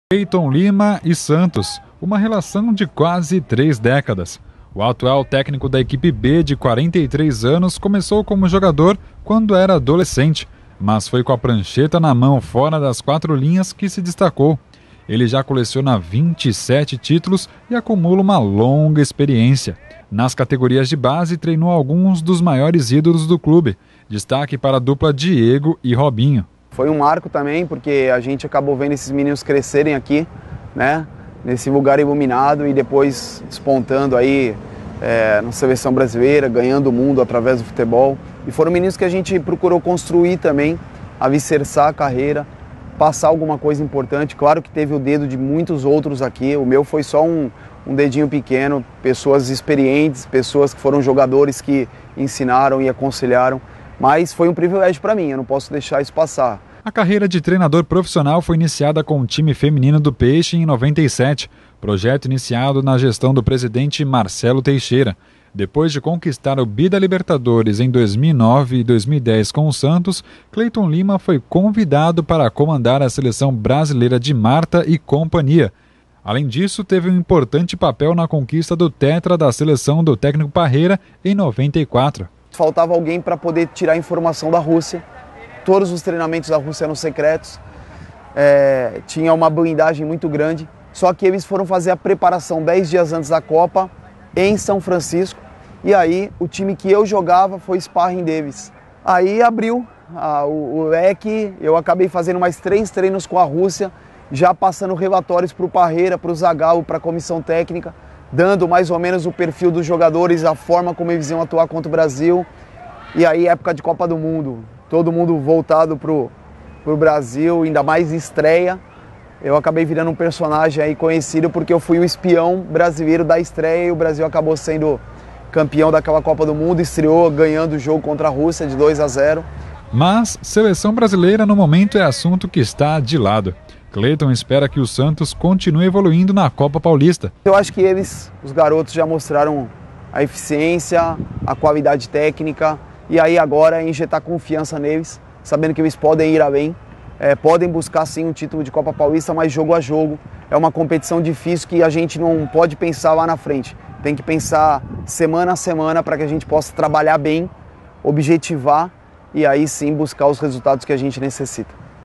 Conheça Kleiton Lima, o trunfo do Santos B
O  Santos B segue treinando para a próxima fase da Copa Paulista. O  time enfrenta o São Caetano no domingo(24) às 22h na Vila Belmiro. A Santa Cecília TV transmite com exclusividade o jogo ao vivo.
E no banco de reservas do peixe, um trunfo: o técnico Kleiton Lima. Ele já treinou alguns dos principais ídolos da equipe e continua revelando novos jogadores no Santos B.